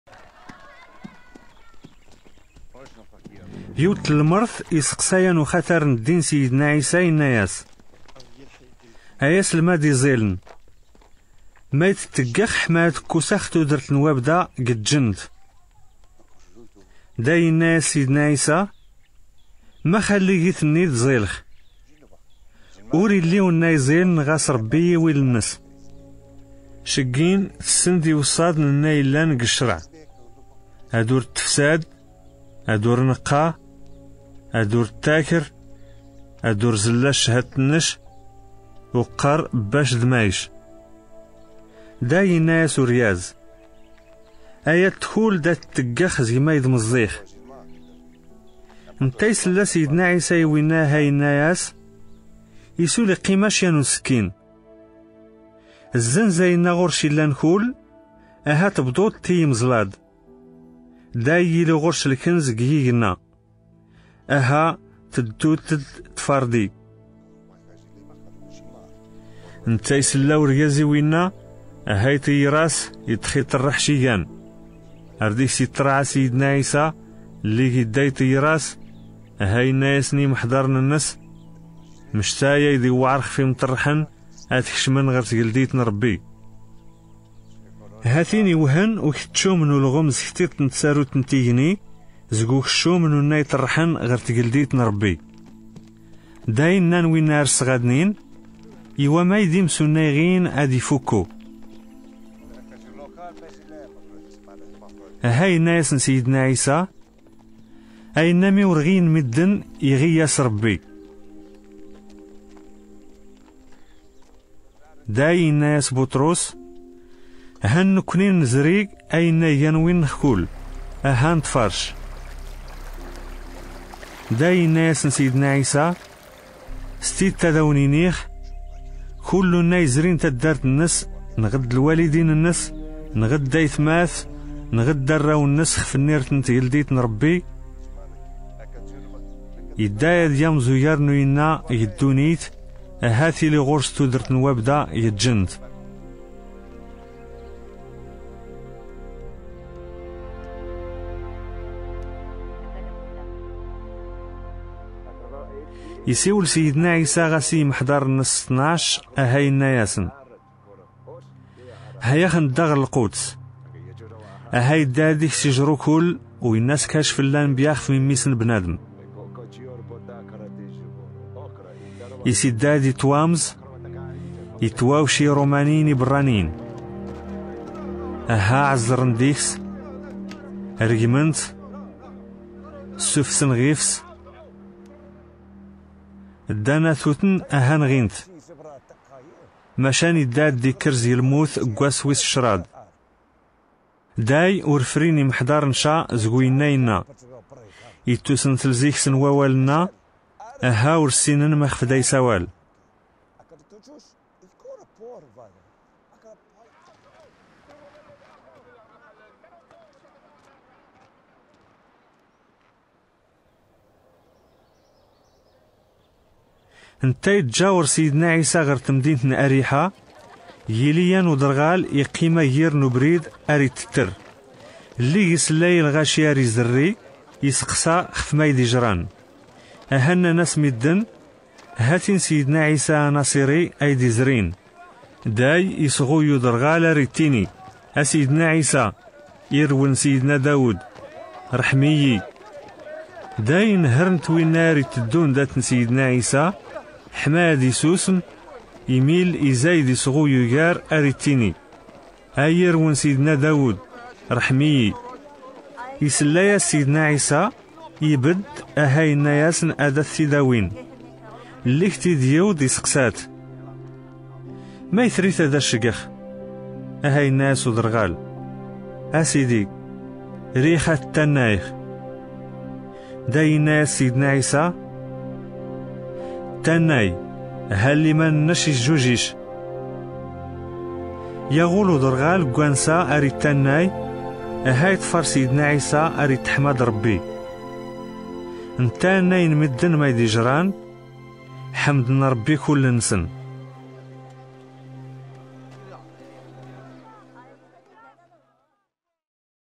Tamazight, Central Atlas | Luke 18:18-43
Lctab n Rebbi s Tamazight n Atlas